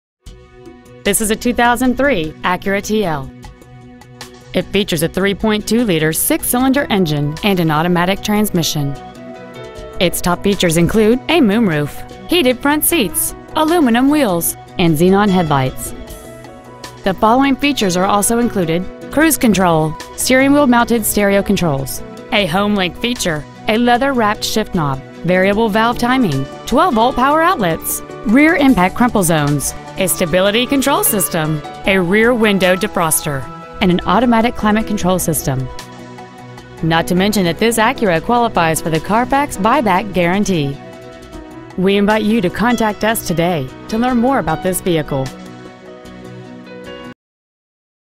Used 2003 Acura TL Sylvania OH
Year : 2003
 Make : Acura
 Model : TL
 Engine : 3.2L SOHC PGM-FI 24-valve V6 engine
 Trans . : Automatic
 Exterior : Black
 Miles : 105,261
 Interior : Other